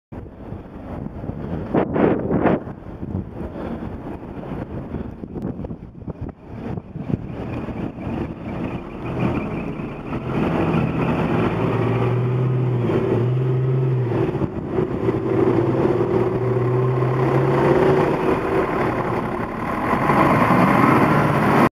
FJ08KLS - National Express - SMIDGAF No indicators
The coach just pulled out on the cyclist in front, without a mirror check or an indication he was going to move. Held us both up, as well as the line of cars following.

Shoddy driving.